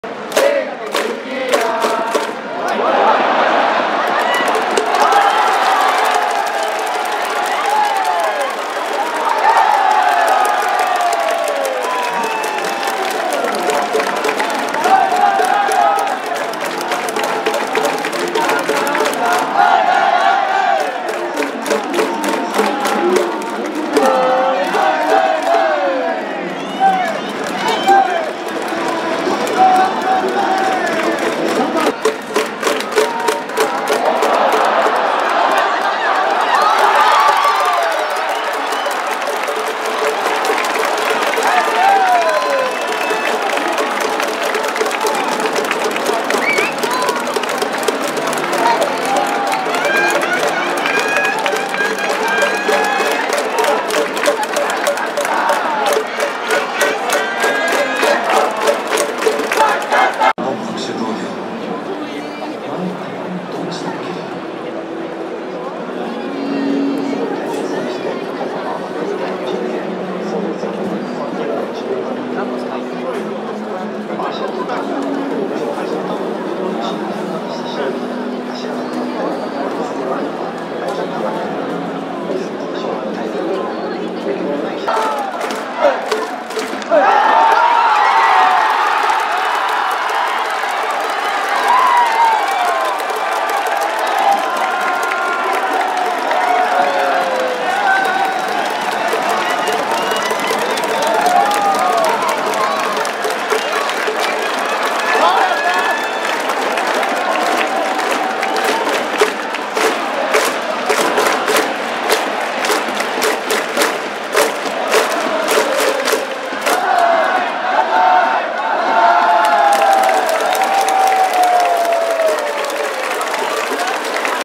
【立浪マジック】打席前にアドバイス送った高橋周平・村松が直後にヒットを放ちベンチで大喜びの立浪監督 2024.4.12
2024年4月12日 中日ドラゴンズ対阪神タイガース in バンテリンドーム